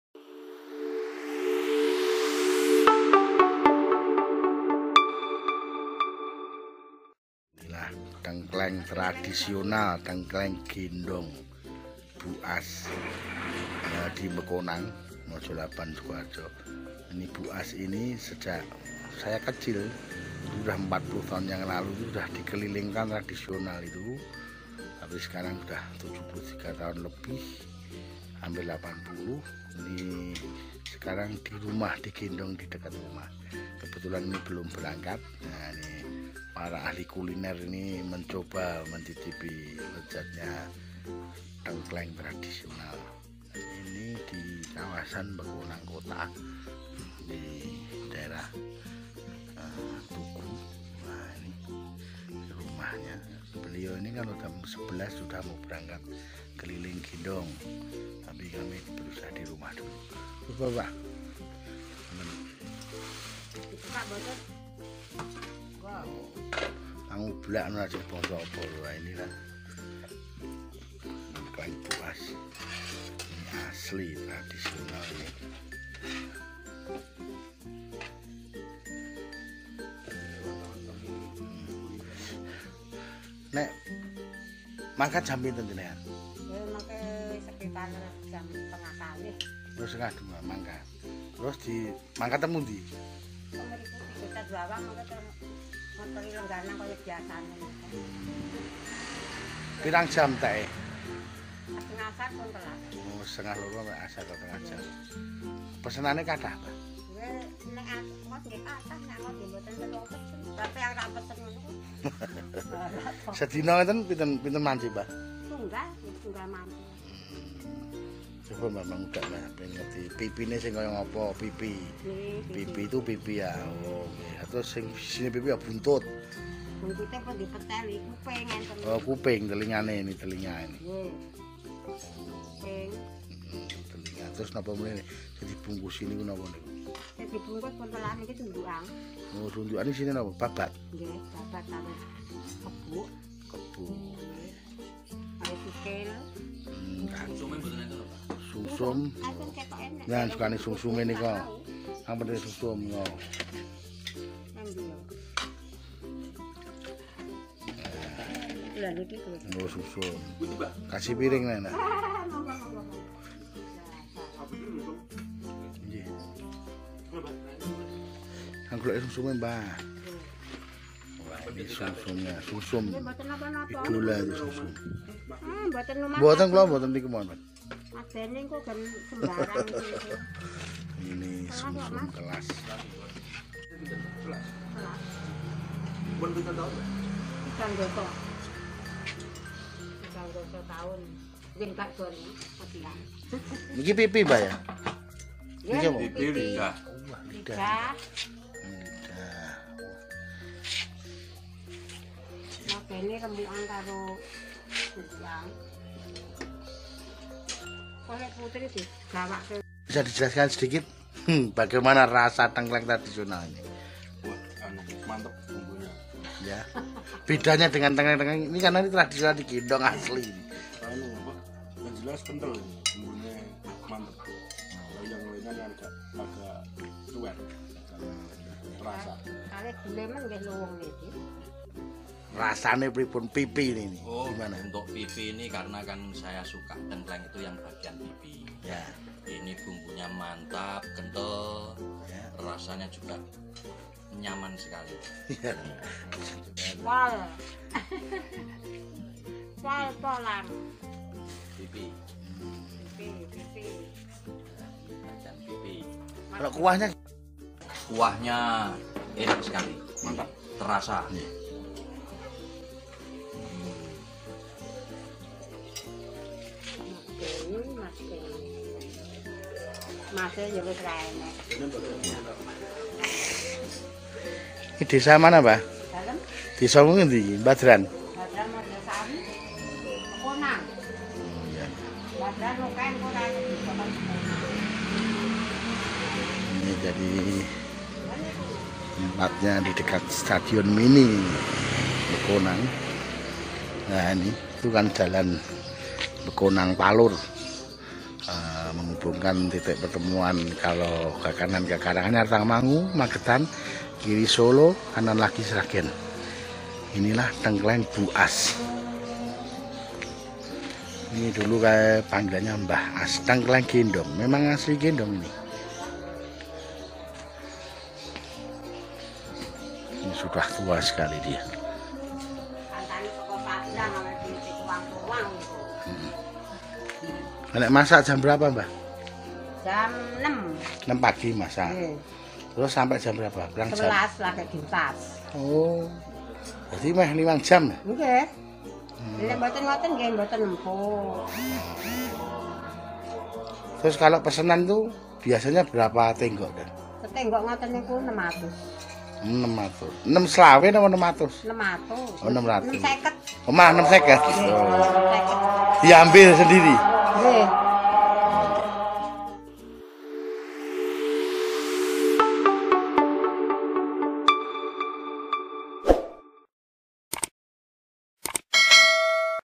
Masakan Legendaris Tengkleng Gendong Bu As, Bekonang, Mojolaban, Sukoharjo, Jawa Tengah
Tengkleng Bu As ini hingga sekarang masih eksis digendong meski usianya sudah 83 tahun. Sedangkan dua sate legendaris Pak Parno dan Bu Parwi juga masih eksis, yang sekarang  diteruskan oleh anak-anaknya.

Tengkleng gendong Bu As patut menjadi rujukab, karena cukup fenomenal.  Selain rasanya dahsyat lezat juga pelanggannya yang banyak. Bahkan saking larisnya tengkleng Bu As  hanya butuh waktu tak sampai dua jam saja selalu ludes diserbu pembeli.

Menurut Bu As karena sudah tua dirinya jualan hanya keliling jarak dekat saja yakni didekat rumahnya di dekat  toko cat sebelah selatan Stadion Mini Bekonang, Mojolaban, Sukoharjo.

Sebelumnya, atau sekitar  40 tahun lalu, setiap hari Bu As berjualan keliling di kawasan Bekonang menempuh jarak hingga 15 kilometer. Jerih payah yang ia lakukan semasa muda, kini berbuah manis.

Nyatanya, di usia senja, ketika ia tak lagi kuat berjalan jauh,  pelanggannya tetap berdatangan.  Bahkan para perwira polisi di Soloraya, instansi pemerintah hingga Bupati jadi pelanggannya.

Silakan coba sendiri datang ke Bekonang sebagai kawasan sate dan tengkleng dijamin pasti huenak, nyamleng.